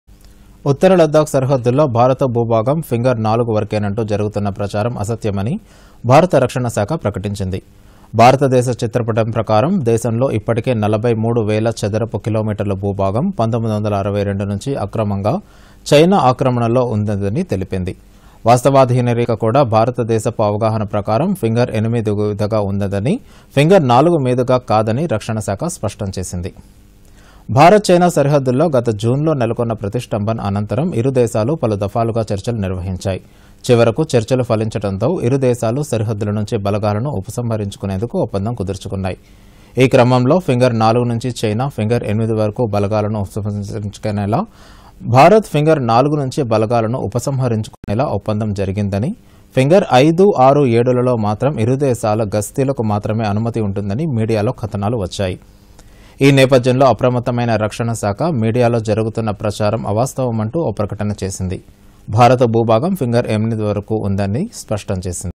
Indian Defense Ministry: Finger 4 of Indian Territory in Northern Ladakh belongs to India
ఉత్త‌ర లద్దాక్ స‌రిహ‌ద్దుల్లో భార‌త భూభాగం ఫింగ‌ర్ 4 వ‌ర‌కేనంటూ జ‌రుగుతున్న ప్ర‌చారం అస‌త్య‌మ‌ని భార‌త ర‌క్ష‌ణ‌శాఖ  ప్ర‌క‌టించింది. భార‌త‌దేశ చిత్ర‌ప‌టం ప్రకారం దేశంలో ఇప్ప‌టికే 43 వేల చ‌ద‌ర‌పు కిలోమీట‌ర్ల భూభాగం 1962 నుంచి అక్ర‌మంగా చైనా ఆక్ర‌మ‌ణ‌లో ఉన్న‌ద‌ని తెలిపింది. వాస్త‌వాధీన రేఖ కూడా భార‌తదేశ‌పు అవగాహ‌న ప్ర‌కారం ఫింగ‌ర్ 8 మీదుగా ఉన్న‌ద‌ని, ఫింగ‌ర్ 4 మీదుగా కాద‌ని ర‌క్ష‌ణ‌శాఖ స్ప‌ష్టంచేసింది.
భార‌త‌-చైనా స‌రిహ‌ద్దుల్లో గత జూన్‌లో నెల‌కొన్న ప్ర‌తిష్టంభ‌న అనంత‌రం ఇరుదేశాలు ప‌లు ధ‌పాలుగా చ‌ర్చ‌లు నిర్వ‌హించాయి. చివ‌ర‌కు చ‌ర్చ‌లు ఫ‌లించ‌డంతో ఇరుదేశాలు స‌రిహ‌ద్దుల నుంచి బ‌ల‌గాల‌ను ఉప‌సంహ‌రించుకునేందుకు ఒప్పందం కుదుర్చుకున్నాయి. ఈ క్‌ామంలో ఫింగ‌ర్ 4 నుంచి చైనా ఫింగ‌ర్ 8 వ‌ర‌కు బలగాలను ఉపసంహరించుకునేలా... భార‌త్ ఫింగ‌ర్ 4 వ‌ర‌కు బ‌ల‌గాల‌ను ఉప‌సంహ‌రించుకునేలా ఒప్పందం జ‌రిగింద‌ని, ఫింగ‌ర్ 5, 6, 7ల‌లో మాత్రం ఇరుదేశాల గ‌స్తీల‌కు మాత్ర‌మే అనుమ‌తి ఉంటుంద‌ని మీడియాలో క‌థ‌నాలు వ‌చ్చాయి. ఈ నేప‌థ్యంలో అప్రమత్తమైన రక్షణశాఖ మీడియాలో జ‌రుగుతున్న ప్ర‌చారం అవాస్త‌వ‌మంనటూ ఓ ప్ర‌క‌ట‌న చేసింది. భార‌త భూభాగం ఫింగ‌ర్ 8 వ‌ర‌కు ఉందని స్ప‌ష్టంచేసింది.